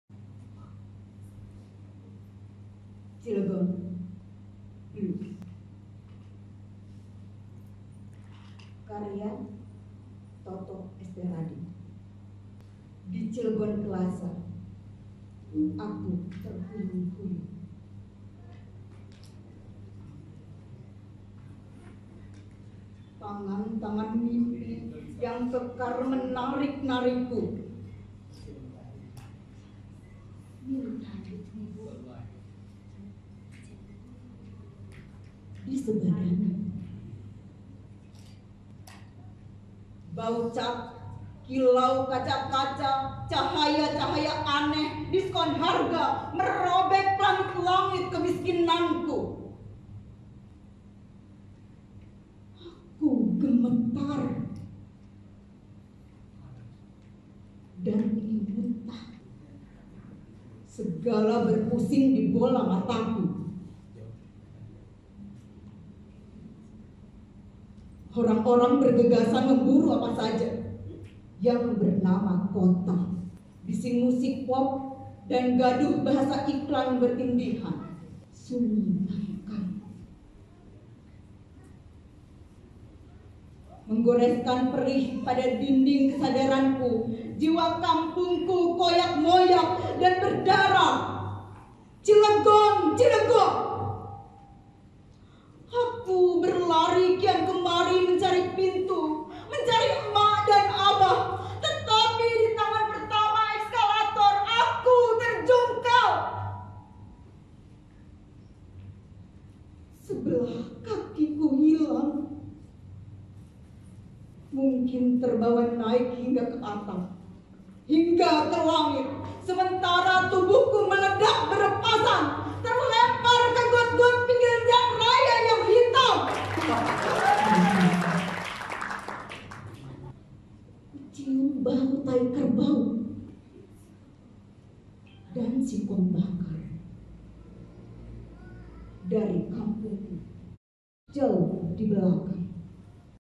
Pembacaan Puisi- Mastra- Cilegon Blues Karya Toto S T Radik oleh Ferra Suharno
Pembacaan Puisi Mastra - Malam Apresiasi Sastra Asia Tenggara - Cilegon Blues Karya Karya Toto S T Radik oleh Ferra Suharno